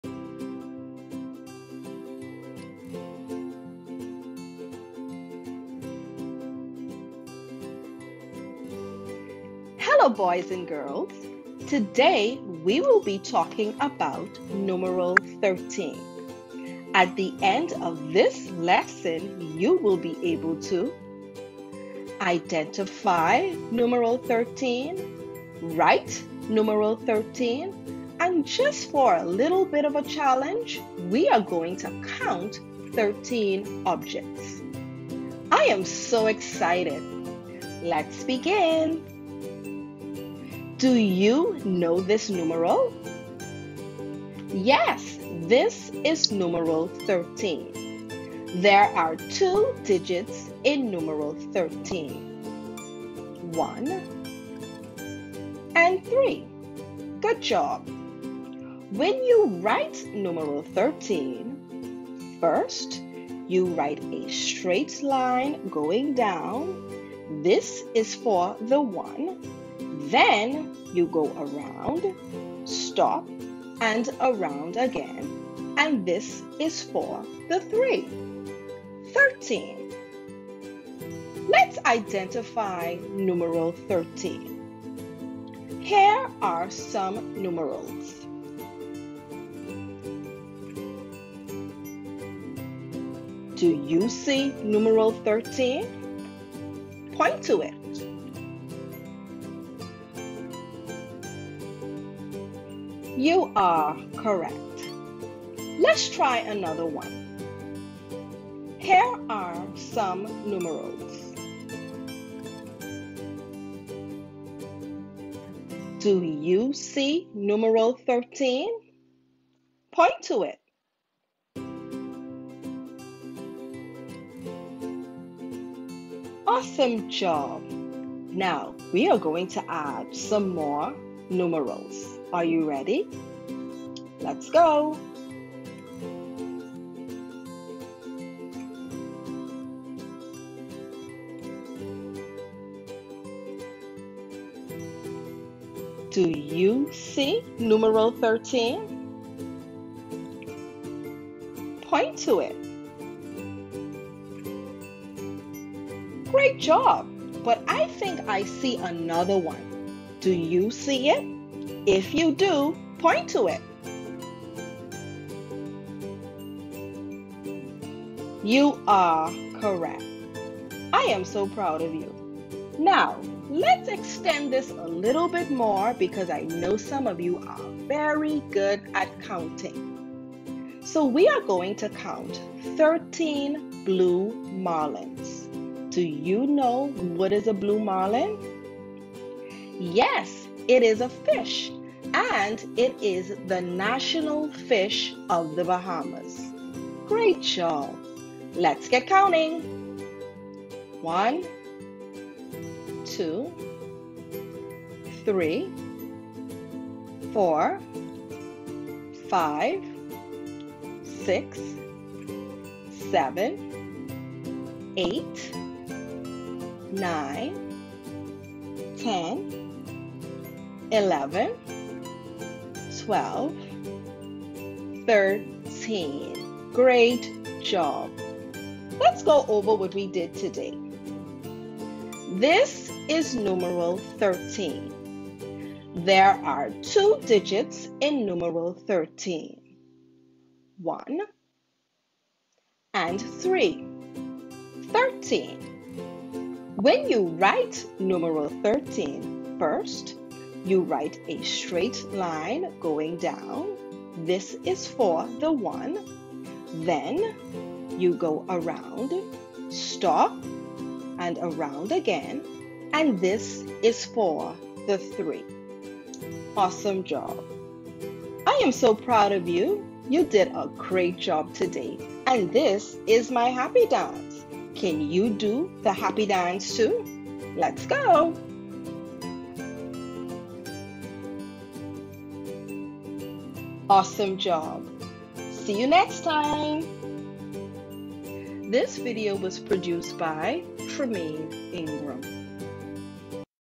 Numeral 13
This video explores identifying, writing and counting to numeral 13.